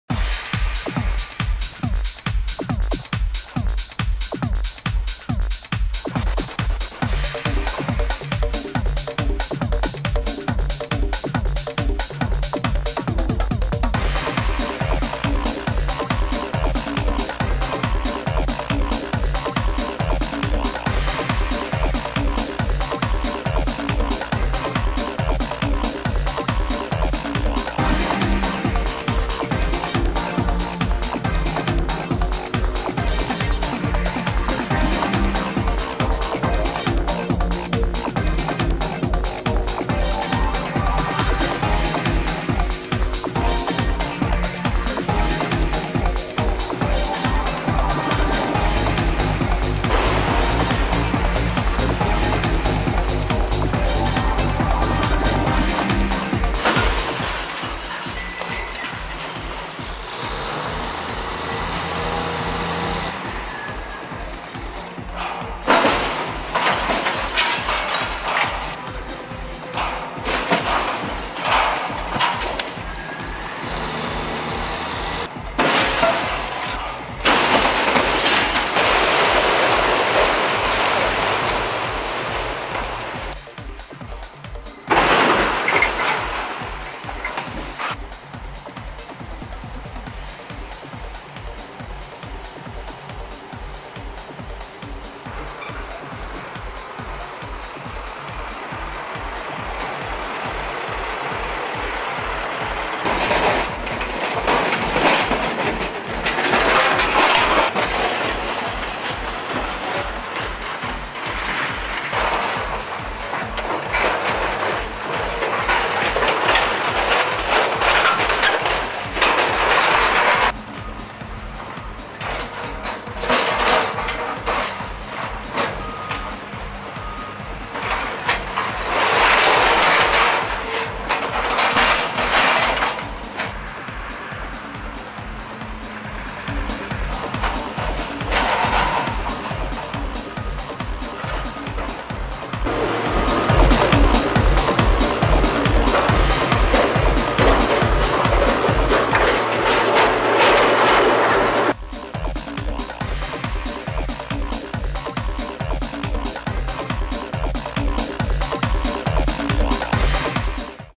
Arnorcast
Arnircast tank factory demolition in Birdsboro, Pa.  Work being completed by Meco Demoltion